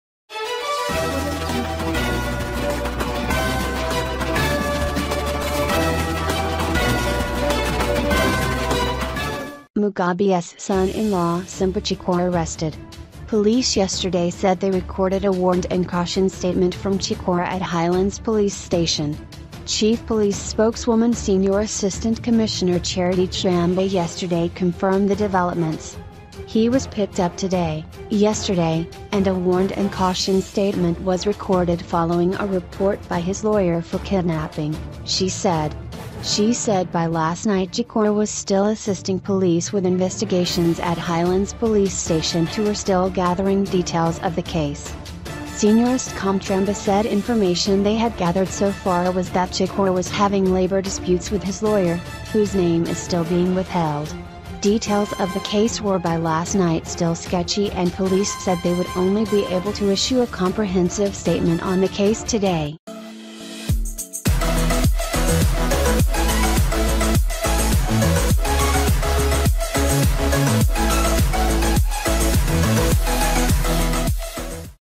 Mugabe ‘s Son In Law Simba Chikore Arrested
Mugabe ‘s Son In Law Simba Chikore Arrested - Former President Mugabe’s son-in-law Simba Chikore was yesterday picked up for questioning by police on allegations of kidnapping his lawyer following an argument over a labour dispute.